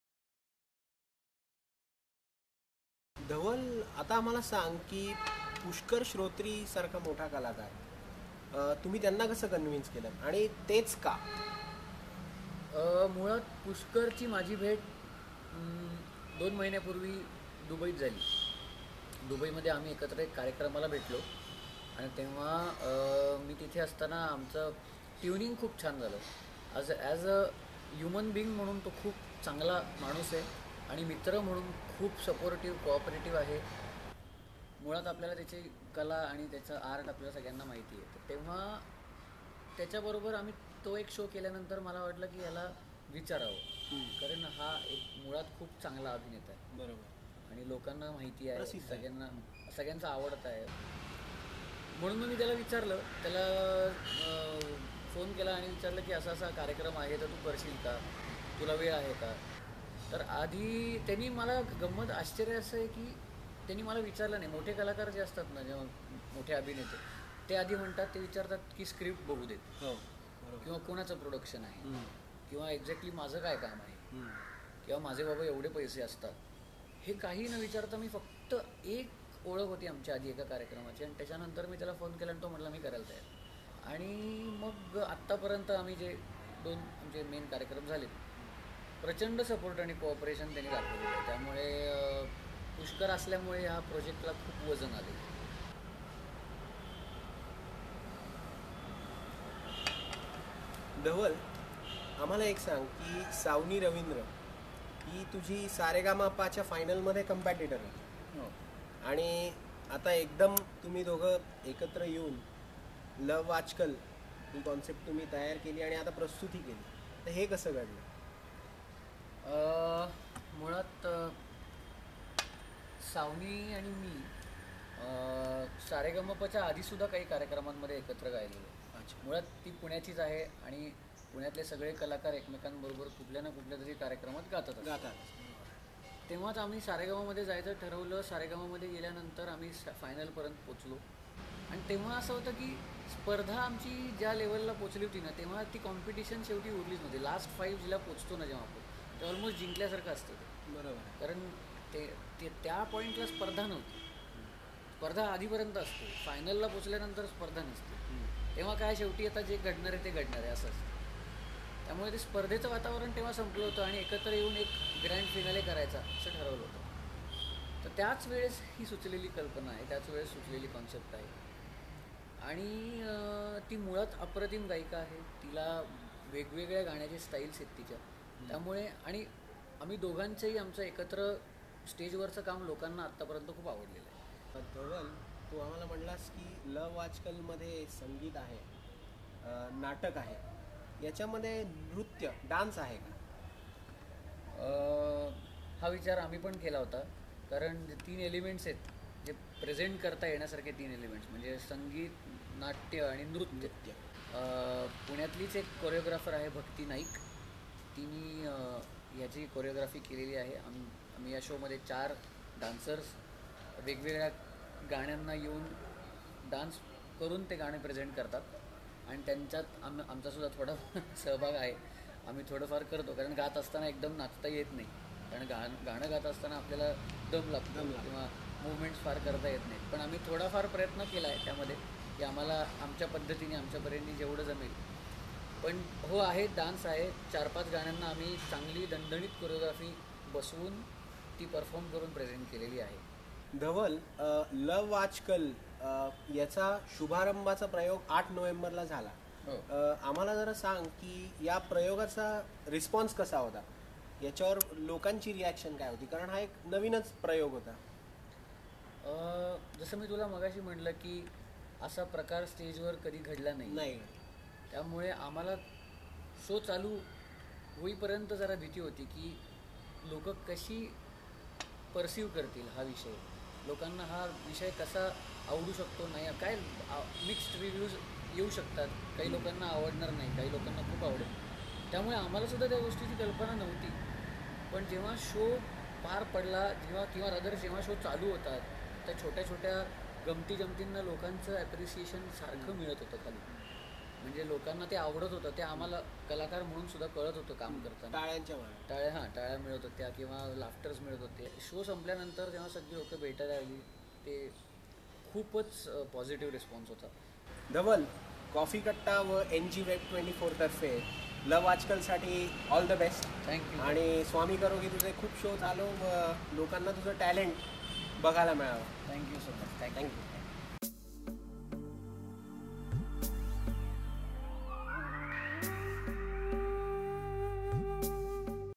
CoffeeKatta with Dhawall Chandawadkkar - Love aaj kaal special - Part 2
Dhawall Chandawadkkar  talks about his new musical play- Love Aaj Kaal